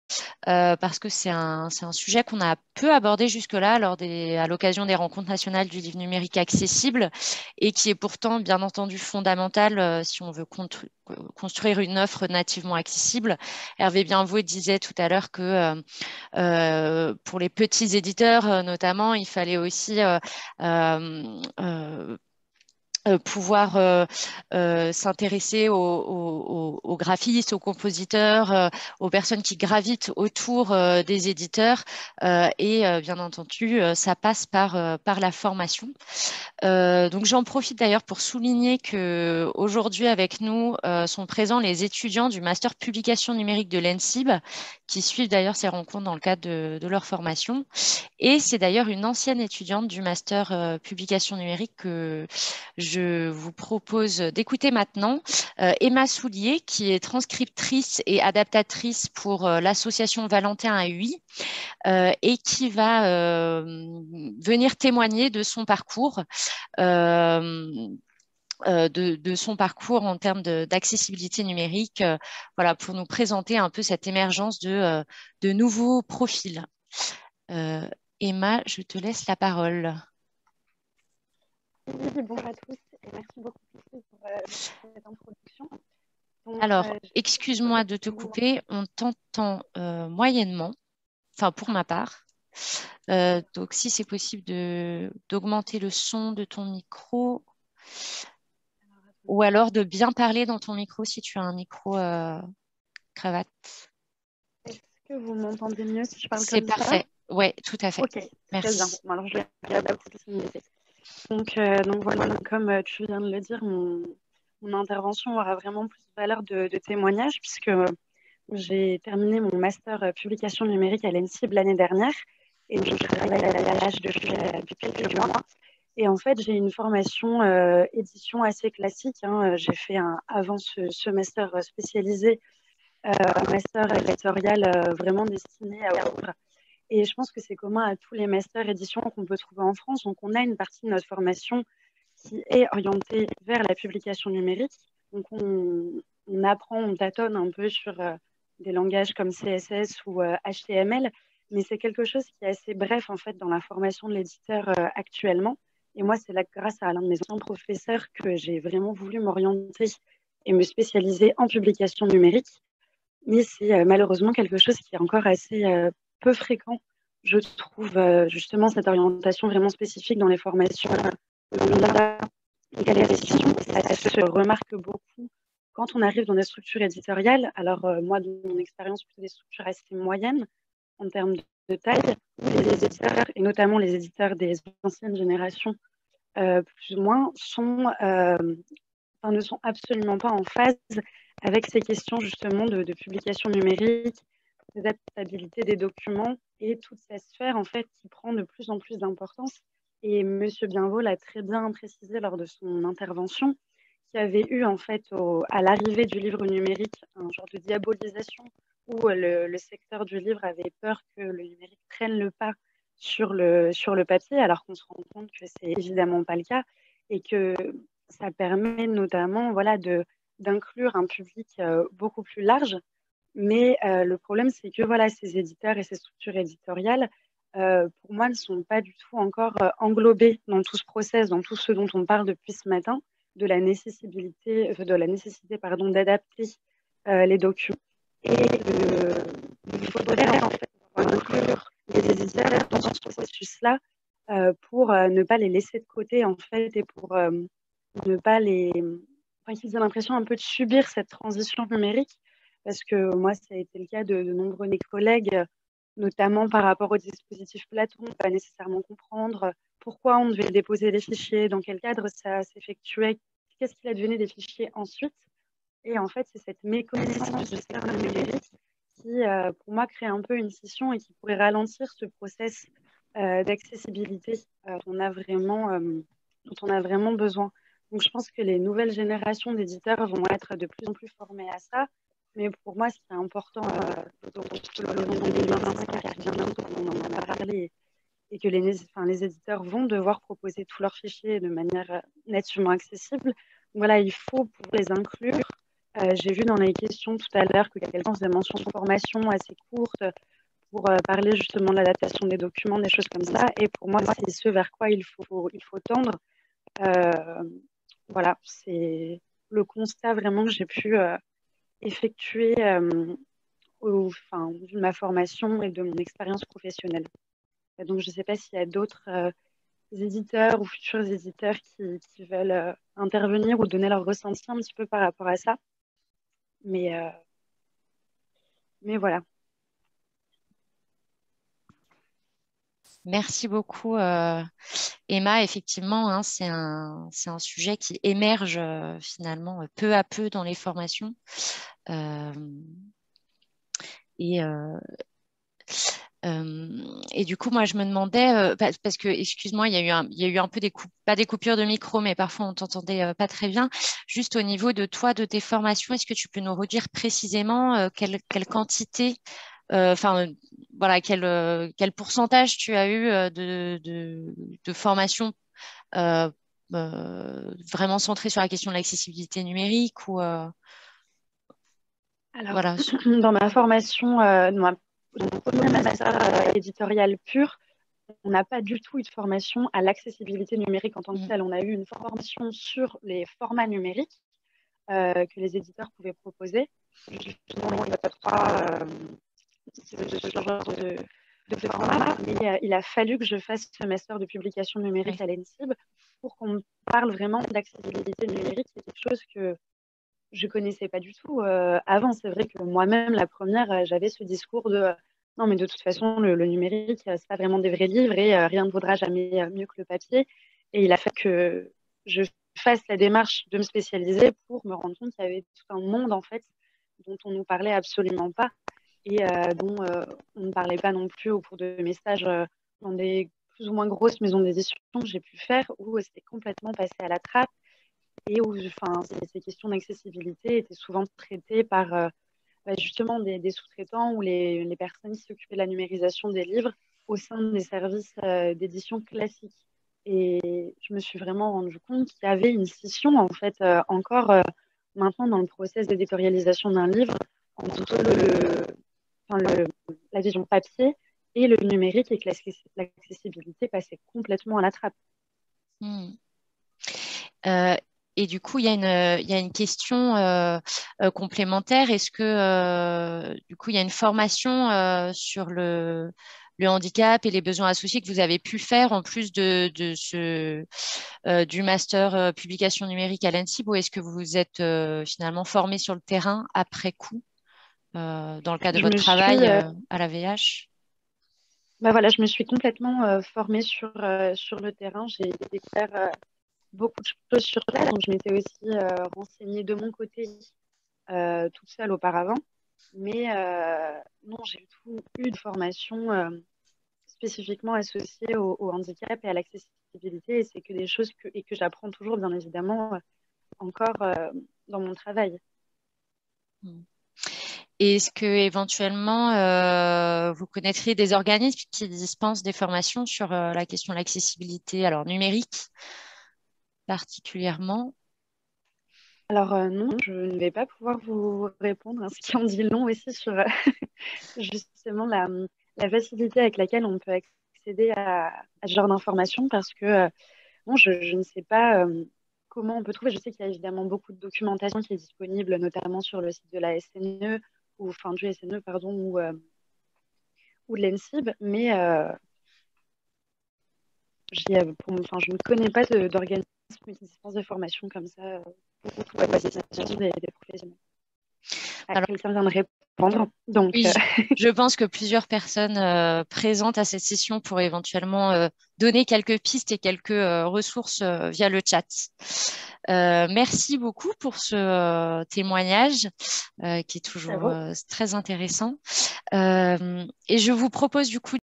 6 - Compétences en accessibilité numérique : émergence et valorisation de nouveaux profils
Témoignage d'Emma Soulier, transcriptrice et adaptatrice pour l'Association Valentin Haüy, dans le cadre des Rencontres nationales du livre numérique accessible (RNLNA) du 20 janvier 2022.